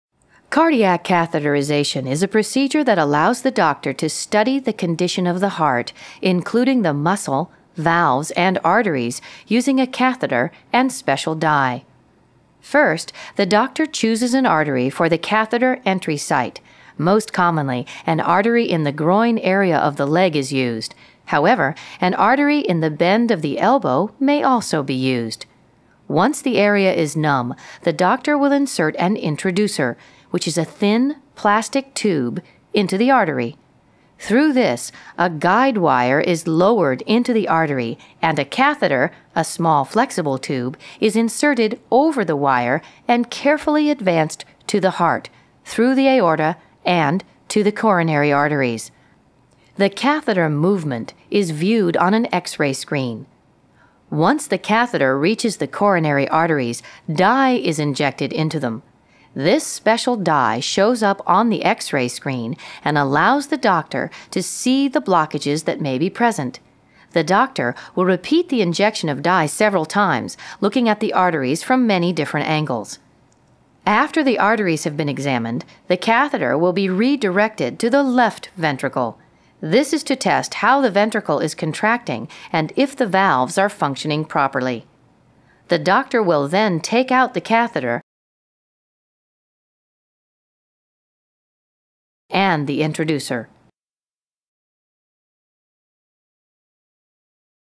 Cardiac Cath
This video clip describes what happens during a cardiac catheterization.  For videos and more check out